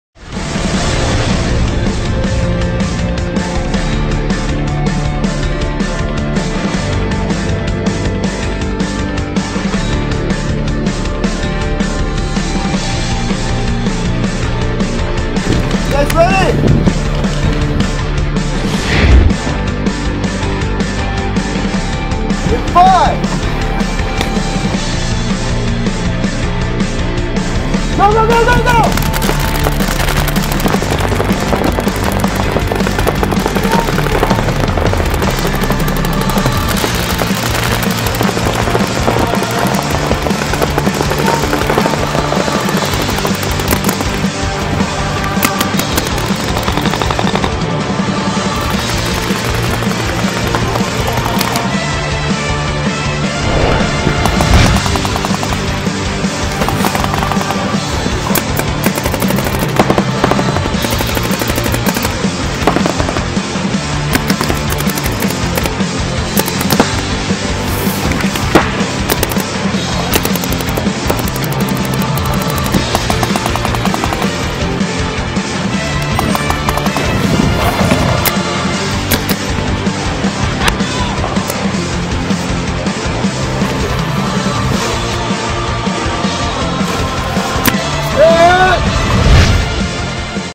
Paintballs and Parkour - Paintball Revolution Factory Team
A little fun demo video with the Paintball Revolution Factory Team at Stryker Paintball in Santa Paula. Side flips over the snake may not be the best way to bunker, but it sure looks fun.